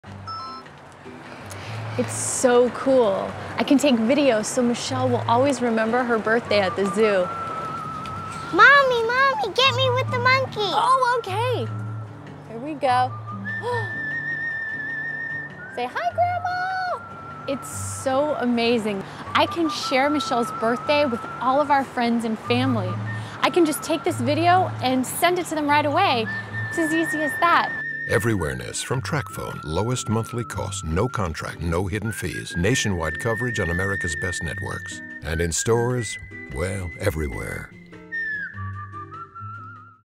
TracFone- Everywhereness Camera
Another installment of the "everywhereness" campaign.  This one promotes the camera function on one of their phones.